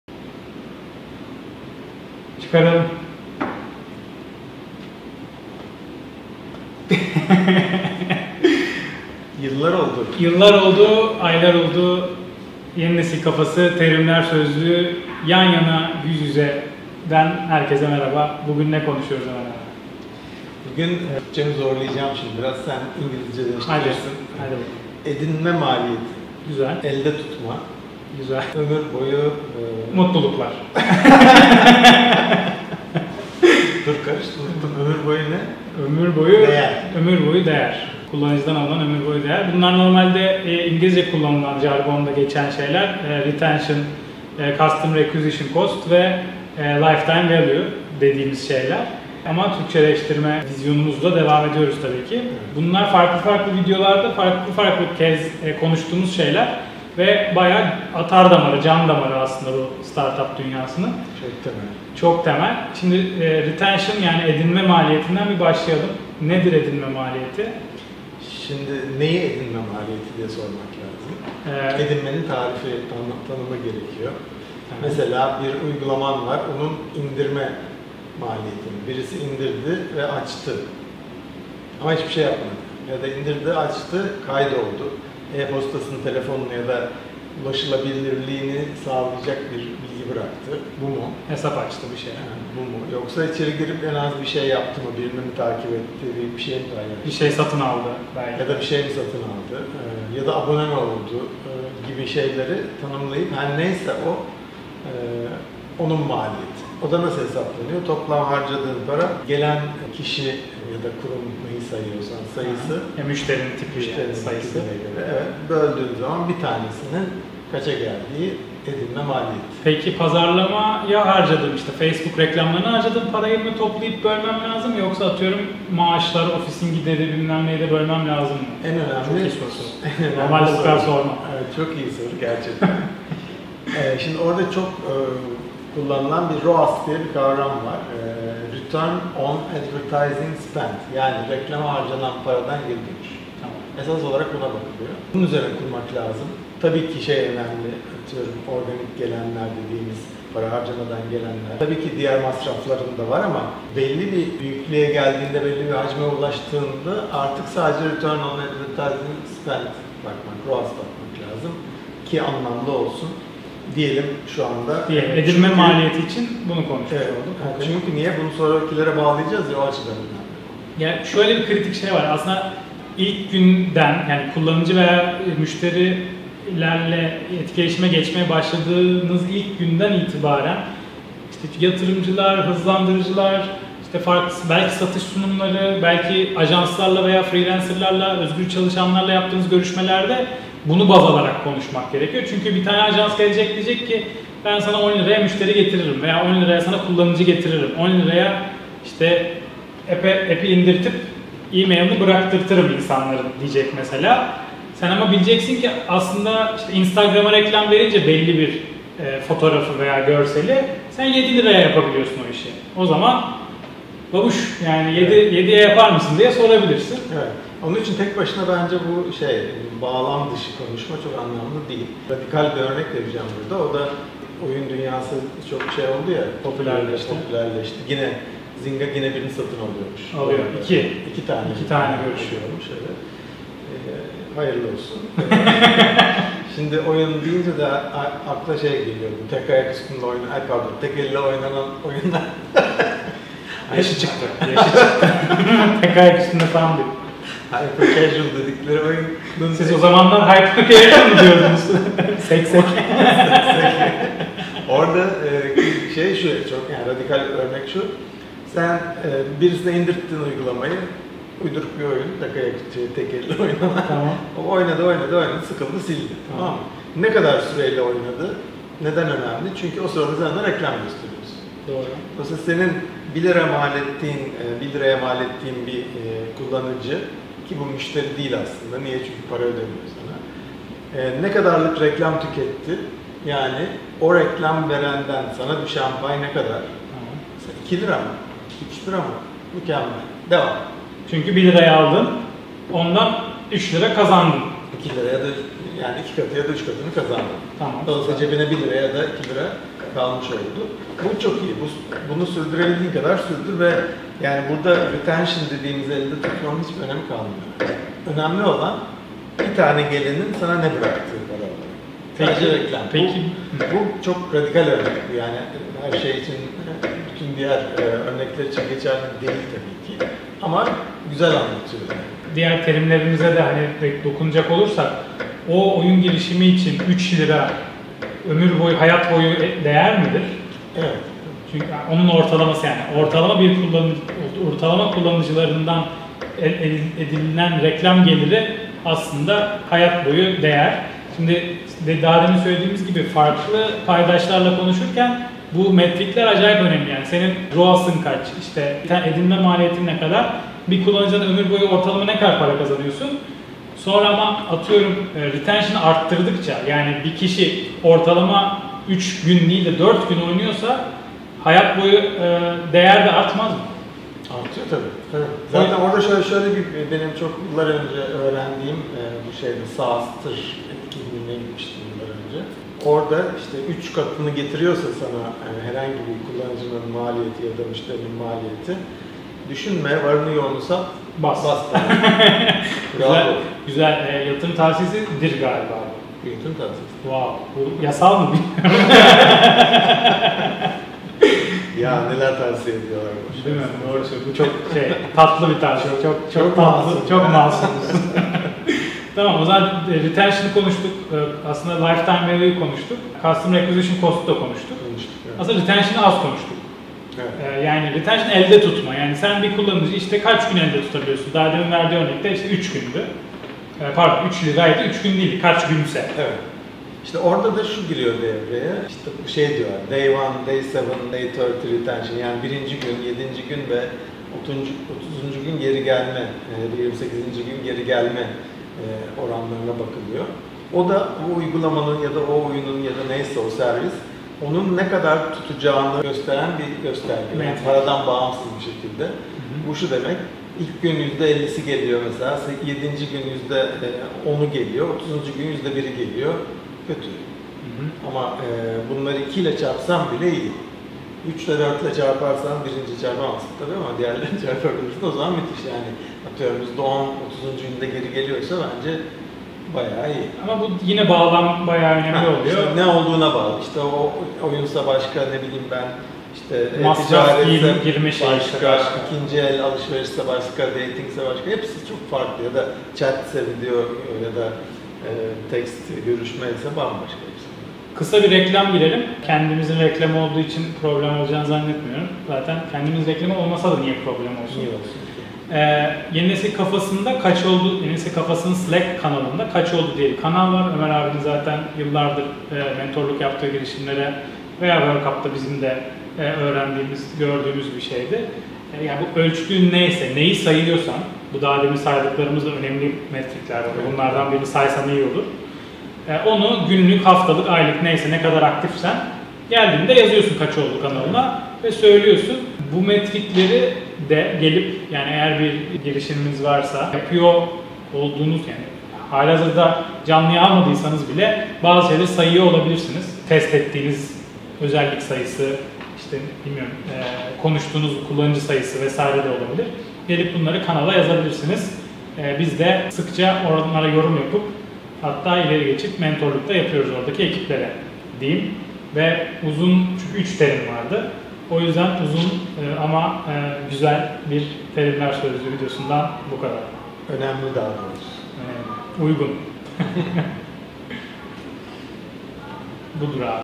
Startup Dünyasının Atar Damarları - “Edinme Maliyeti - Elde Tutmak - Ömür Boyu Değer”
Yeni Nesil Kafası Terimler Sözlüğü'nün bu bölümünde Startuplar için en önemli terimlerden olan "Edinme Maliyeti (Customer Acquisition Cost), Elde Tutmak (Retention) ve Ömür Boyu Değer (Lifetime Value)" kavramlarından bahsetti. İyi seyirler 🥳😎

Girişimcilikte temel kavramlar
Girişimcilik terimleri